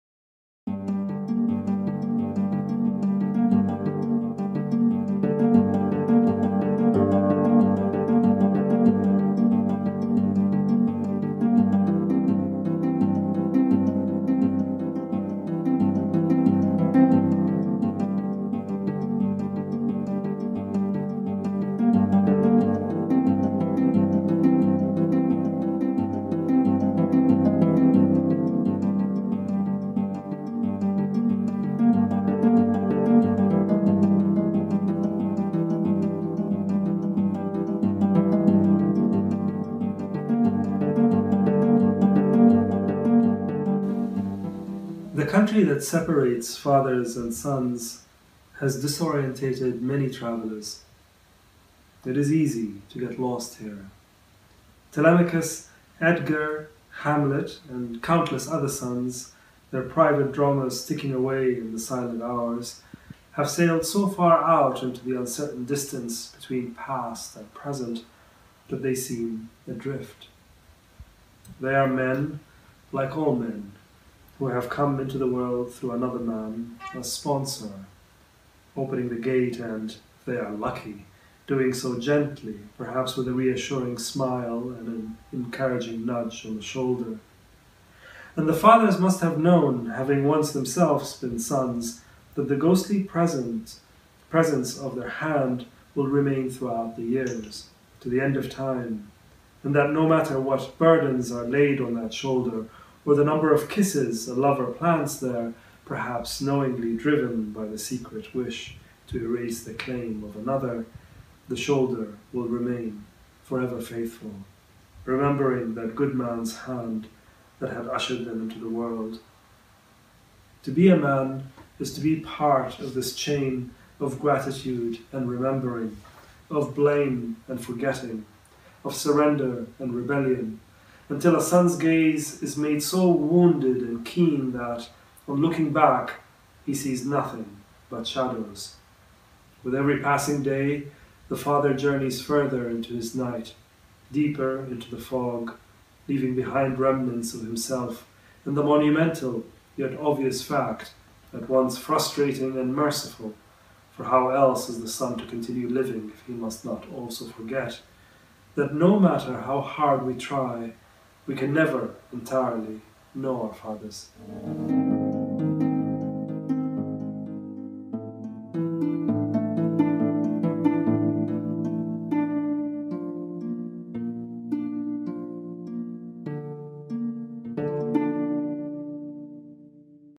Hisham Matar reads from his book 'The Return: Fathers, Son And The land In Between'
Libyan-American writer Hisham Matar's memoir, The Return: Fathers, Son And The land In Between, won the 2017 Pulitzer Prize for Biography and the Rathbones Folio Prize. The book recounts his journey back to Libya in search of his father who was kidnapped and imprisoned by Gaddafi, and never heard from again. Matar reads from the book at his studio in London.